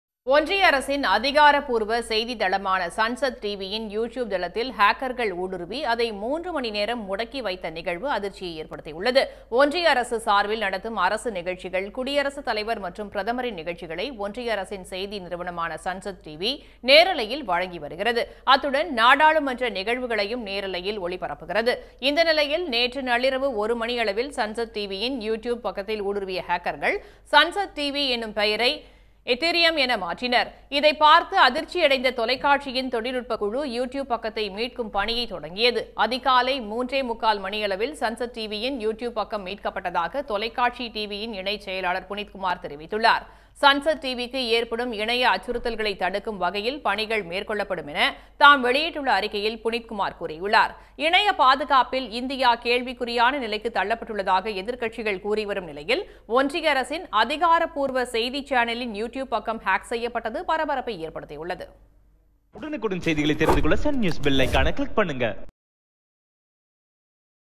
ஒன்றிய அரசின் அதிகாரப்பூர்வ யூடியூப் சேனலான சன்சத் டிவி திடீர் முடக்கத்தால் அதிர்ச்சி | sansad tv
sansadtv | hacking | rstv | ஒன்றிய அரசின் அதிகாரப்பூர்வ யூடியூப் சேனலான சன்சத் டிவி திடீர் முடக்கத்தால் அதிர்ச்சி | sansad tv